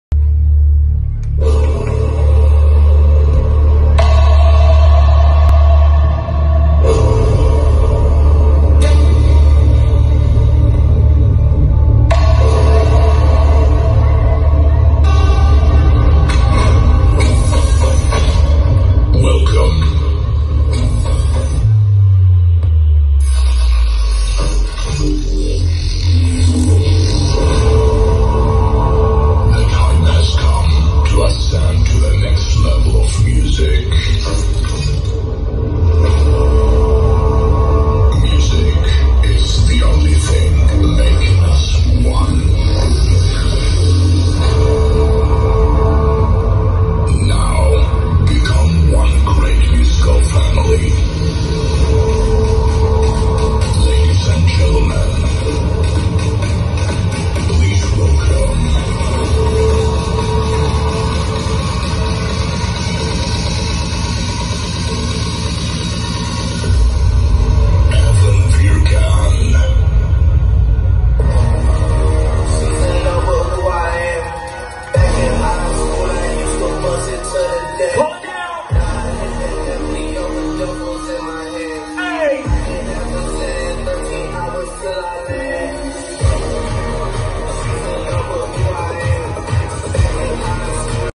STUDIOGAMBARGERAK: MAKING DJ EVAN VIRGAN'S VISUAL INTRO, SPECIAL FOR DWP 2019
Making a special visual intro for DJ @evanvirgan at DWP 2019.. the theme is Sang Kala Bhoma... dope ..!!! 🤘

This is SANG KALA BHOMA that I made in Cinema4D.
In Balinese temple architecture, a Bhoma is a carved or formed grotesque which decorates certain parts in the Balinese temple complex. The statue is similar to the Javanese Kala and was intended to protect the temple complex from a malevolent spirit.